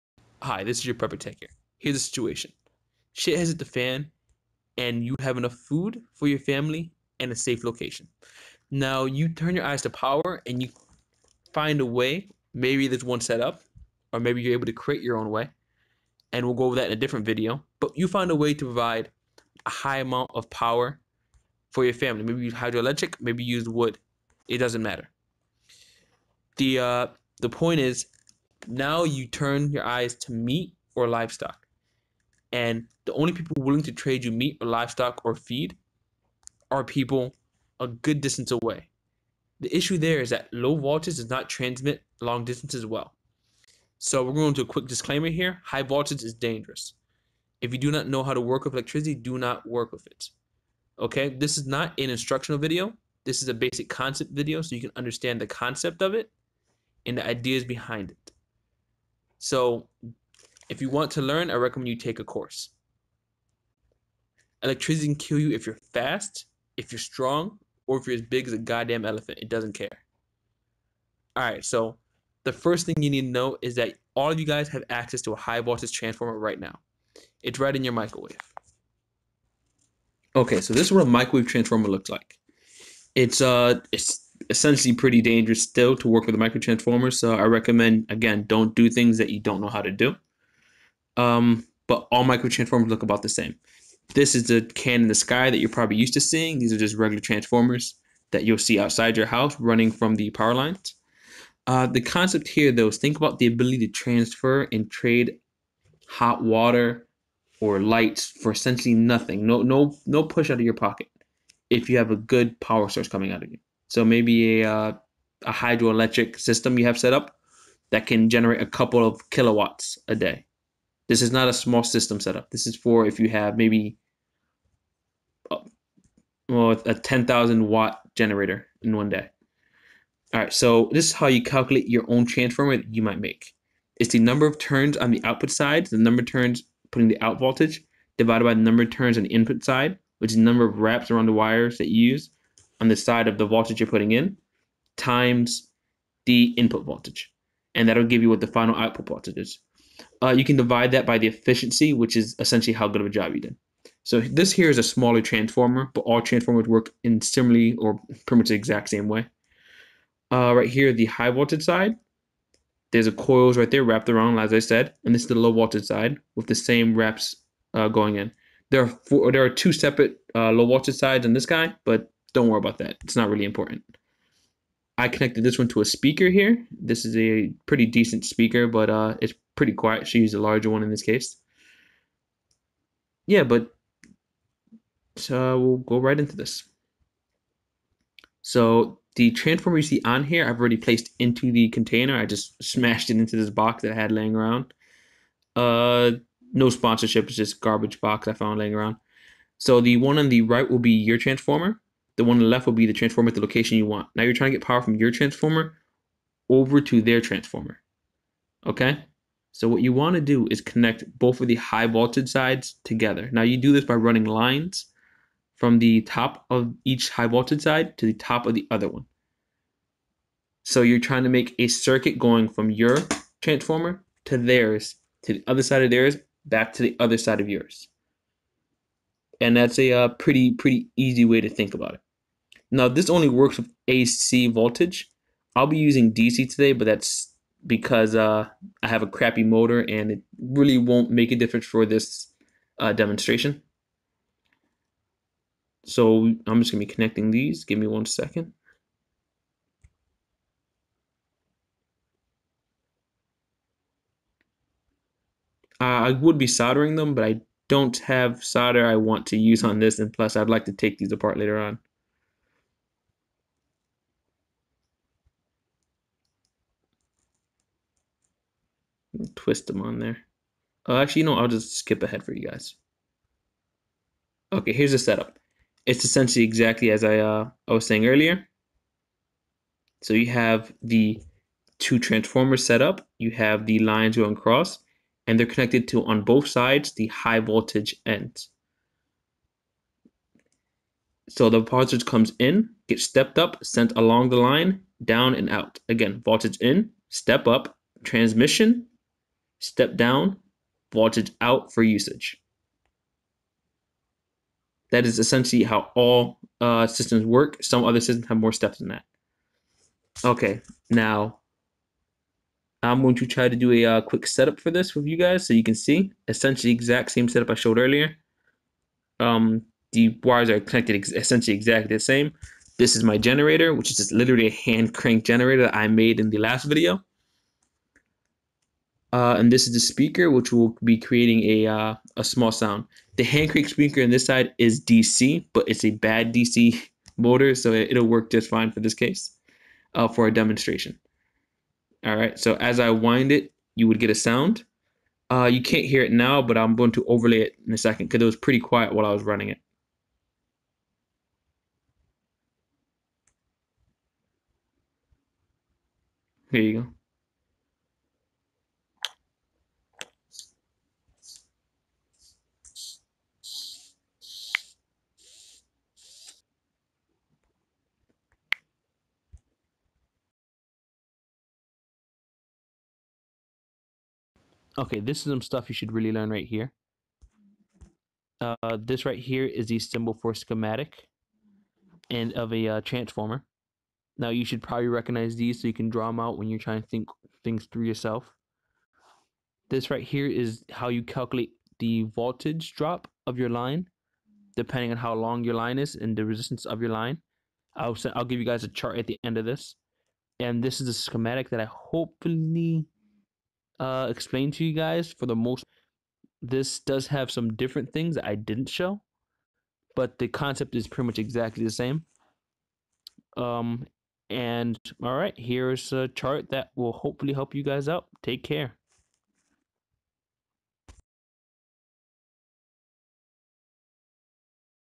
Shtf trading power: watts the cost
The last chart is a wire gauge resistance chart